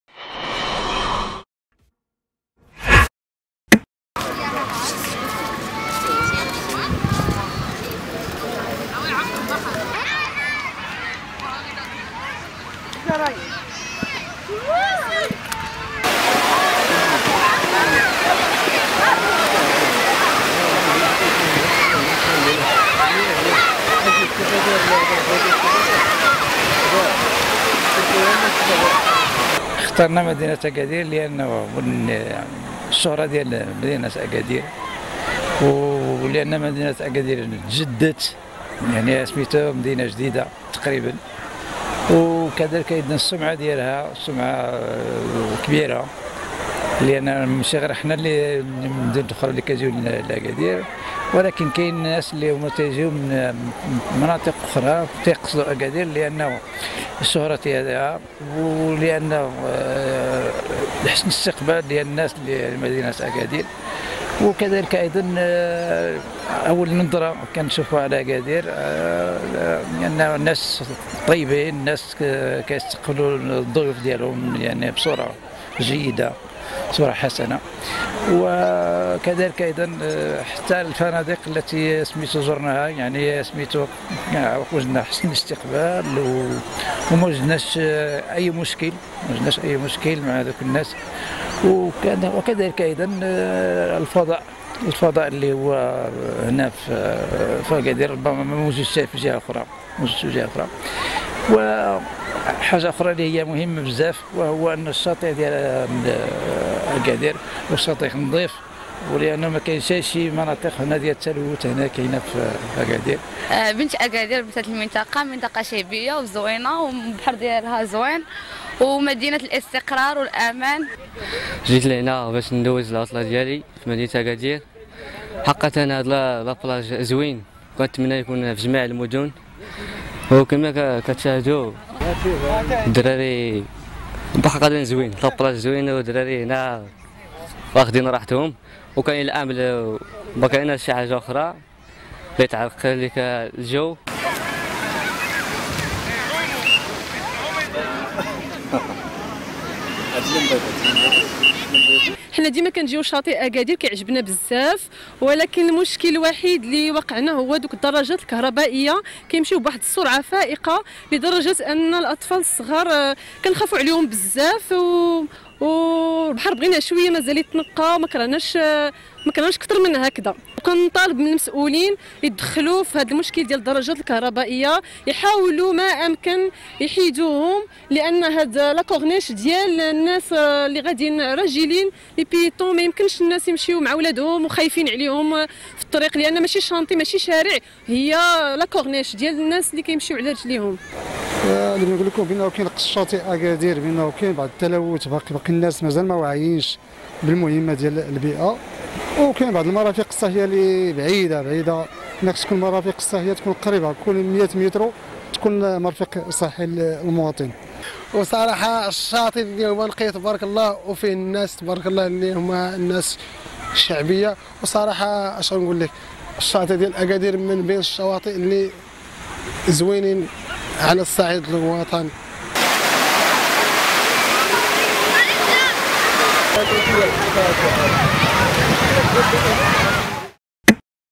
Le360.ma • هذه هي أسباب اختيار المغاربة لشاطئ أكادير للإصطياف وهذه هي مطالبهم
خلال فترة فصل الصيف يختار المغاربة العديد من شواطئ المملكة للاستمتاع بأجوائها الرائعة ولقضاء أوقات ممتعة مع أفراد عائلاتهم، كاميرا le360 تأخذكم في جولة بشاطئ أكادير لمعرفة أسباب اختيار المصطافين لهذا الشاطئ وماذا ينقصه ليكون الأفضل.